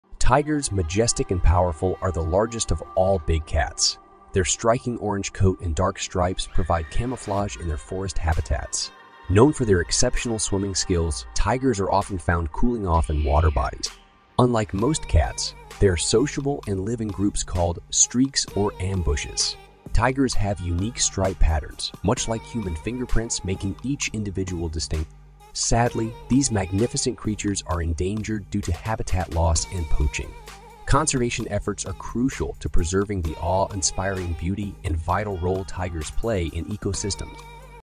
Untamed Marvels: Exploring the Enigma of Tigers 🐅
Unleash the Untamed: Dive into the captivating world of tigers in just seconds! 🐅 Discover spellbinding tiger trivia, from their majestic roars to their secret lives in the wild. Get ready to be whisked away on a wild journey of awe and amazement. Join us for a roarin' good time! 🌟🐾